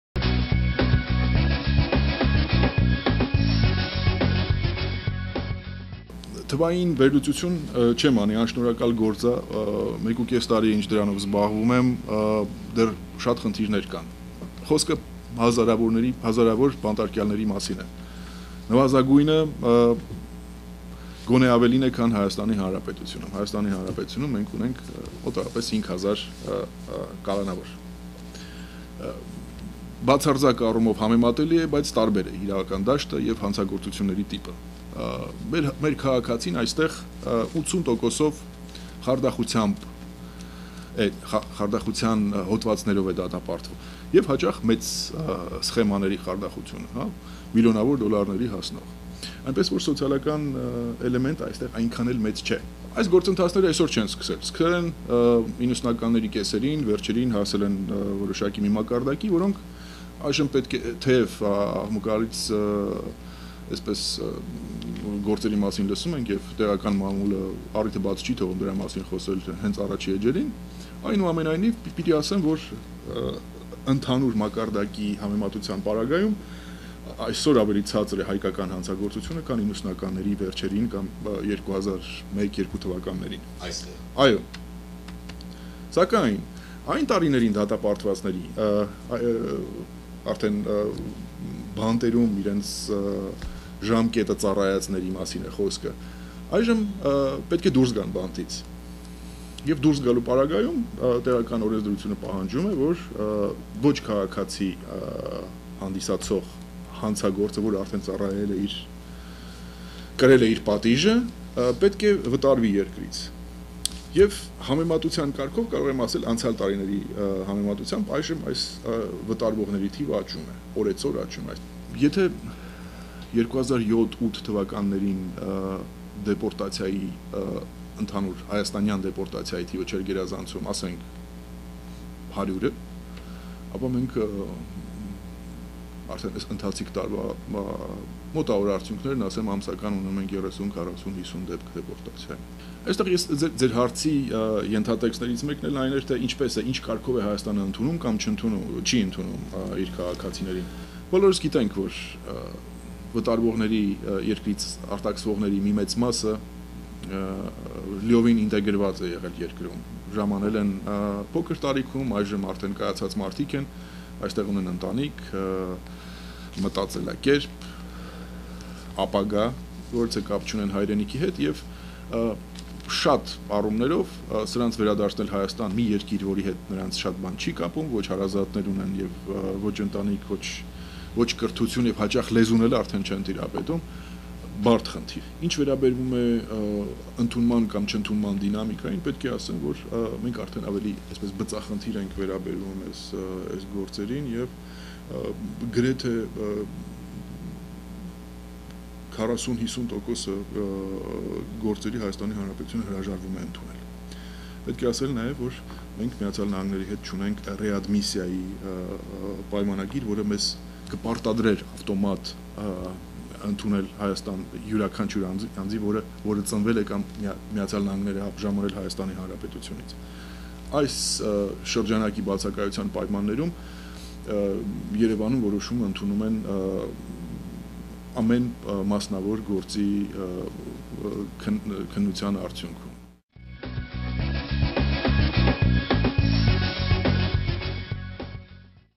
Armen DIlanyan Interviewes Grigor Hovhannissian - PART 3
Consul General  of the Republic of Armenia in Los Angeles  Mr. Grigor Hovhannissian  speaking about the Armenian Diaspora, internal problems of the Armenian-American community and the Republic of Armenia, relations and possible interaction  between diaspora and Armenia, level of  criminality as well as the  problems of deportation of the Armenian citizens.   Interview with Armen Dilanyan. May 22, 2010, ARTN-SHANT TV (USA).